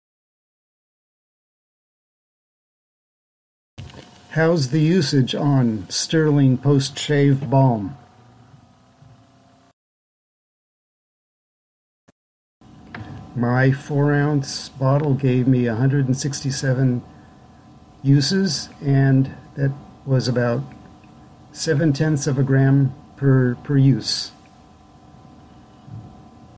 stirling post shave balm usage (revised)
Revised, slightly reorganized, shorter.
Just giving usage results of "how much"